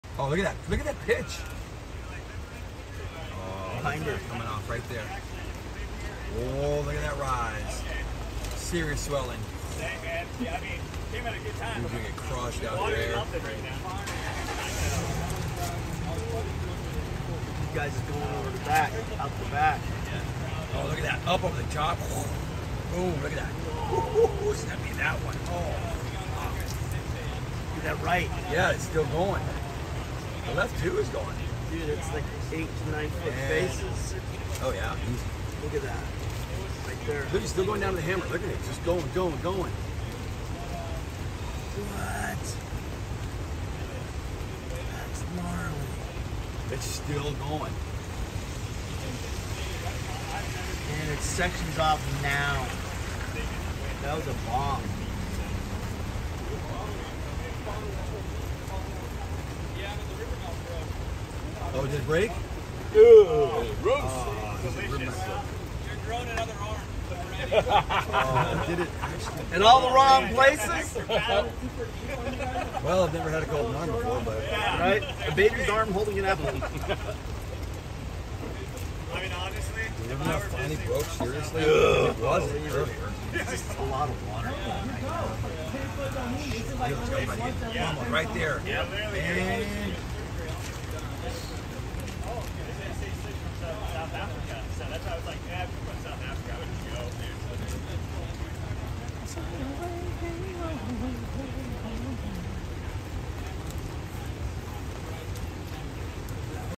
Hurricane Hilary at Doheny State Beach
David Olive capture the hurricane swell on Sunday, Aug. 21, 2023, at Doheny State Beach. The state beach closure didn’t stop these surfers from showing up.